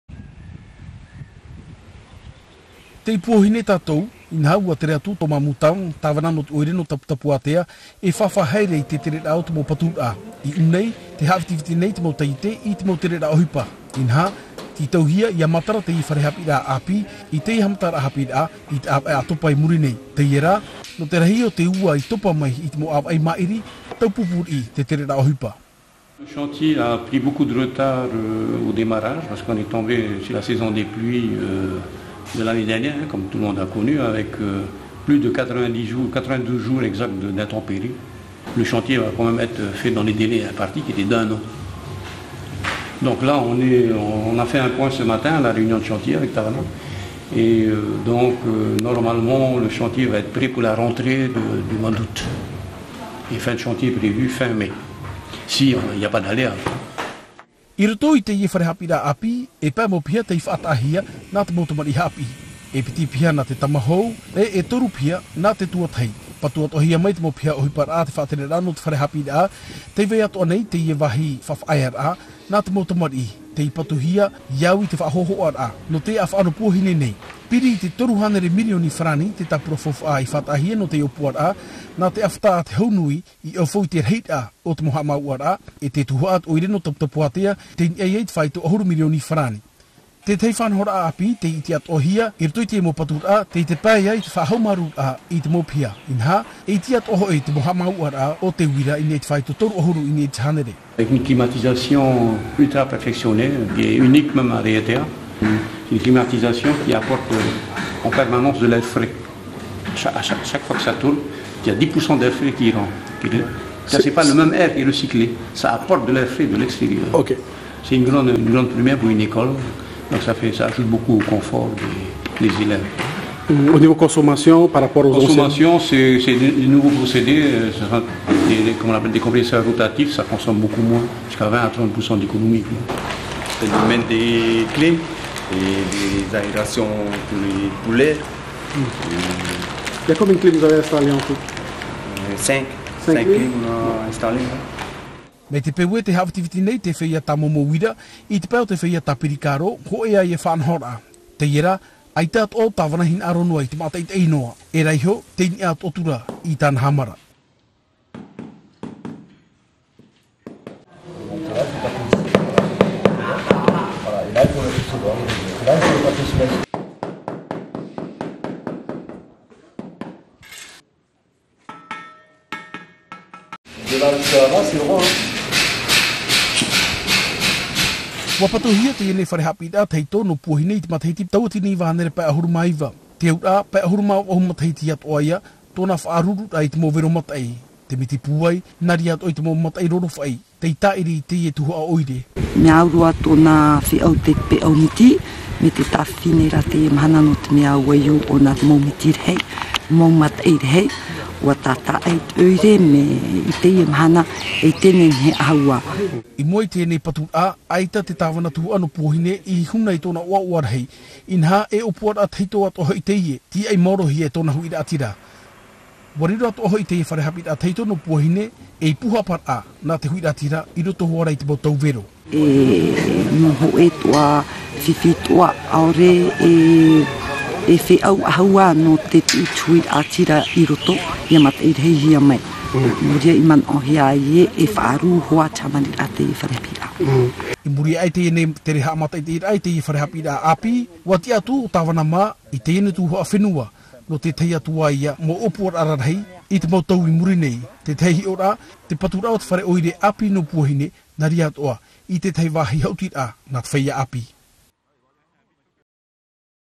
Paturaa api a te fare haapiiraa tuatahi no Puohine
Tere no te Tavana Oire no Taputapuatea, Thomas Moutame no te mata'ita'i i te tere raa ohipa i te pae no te mau paturaa o te fare haapiiraa tuatahi api no Puohine.